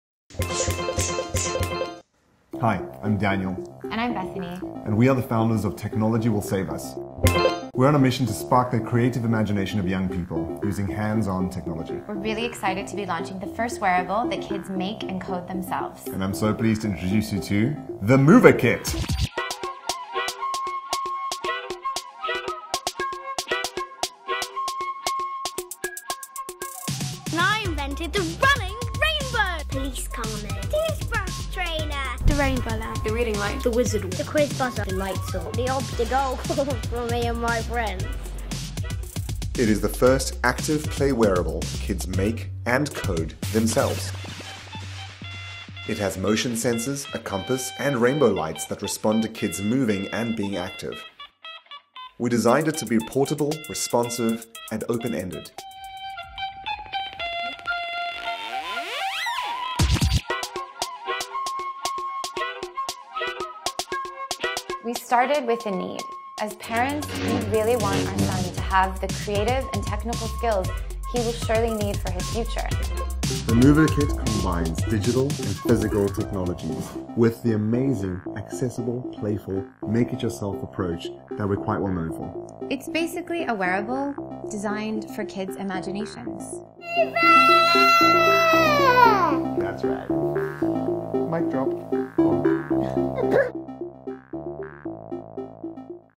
Mover Kit: get kids moving, building & coding
An educational toy kids build themselves that reacts to your movement with light and can be coded in infinite ways!

What is the Mover Kit?

The Mover Kit is an intuitive way for kids ages 8 and over to learn the fundamentals of electronics, programming and solve problems creatively. It encourages kids to learn by doing what they do best - being active and playing!

Right now we are production ready and are turning to you, the Kickstarter community, to get the Mover Kit into the hands of kids everywhere.